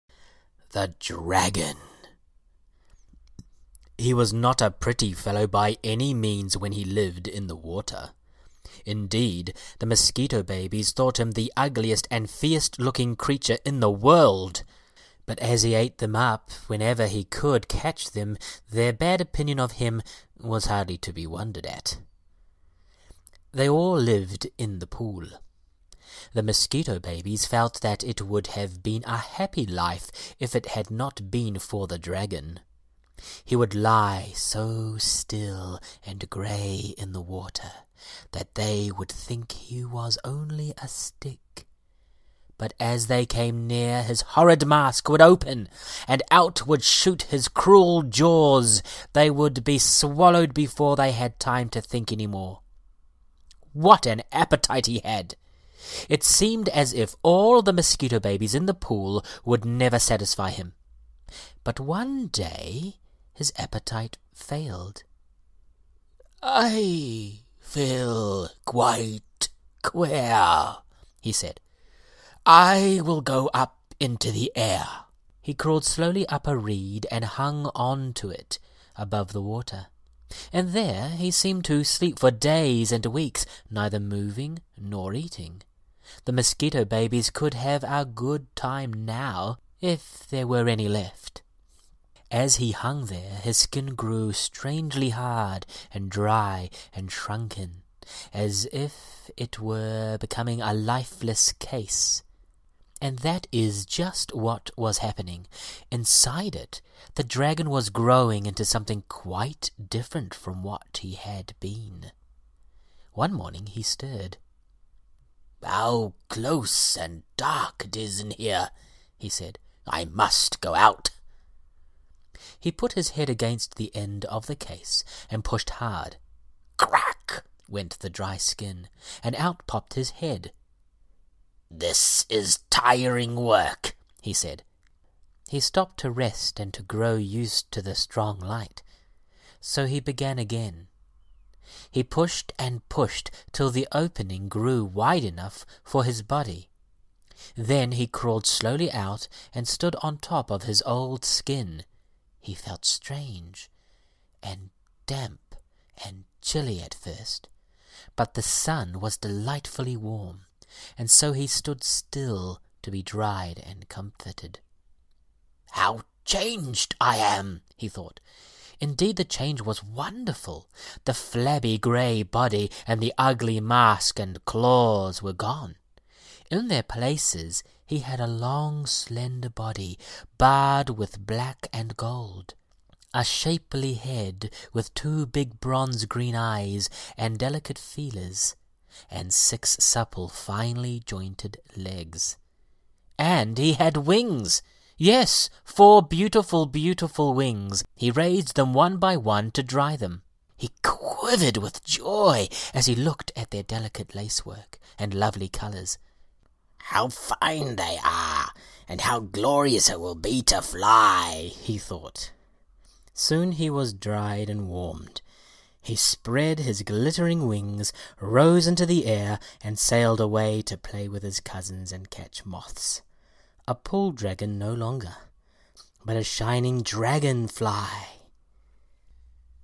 The Dragon by Edith Howes
The Dragon by Edith Howes read Glenn Hibburt

HE was not a pretty fellow by any means when he lived in the water. Indeed, the mosquito babies thought him the ugliest and fiercest-looking creature in the world; but as he ate them up whenever he could catch them their bad opinion of him was hardly to be wondered at.
 They all lived in the pool. The mosquito babies felt that it would have been a happy life if it had not been for the Dragon. He would lie so still and grey in the water that they would think he was only a stick, but as they came near his horrid mask would open, and out would shoot his cruel jaws; they would be swallowed before they had time to think any more. What an appetite he had! It seemed as if all the mosquito babies in the pool would never satisfy him.
 But one day his appetite failed. “I feel very queer,” he said. “I will go up into the air.” He crawled slowly up a reed and hung on to it above the water, and there he seemed to sleep for days and weeks, neither moving nor eating. The mosquito babies could have a good time now—if there were any left.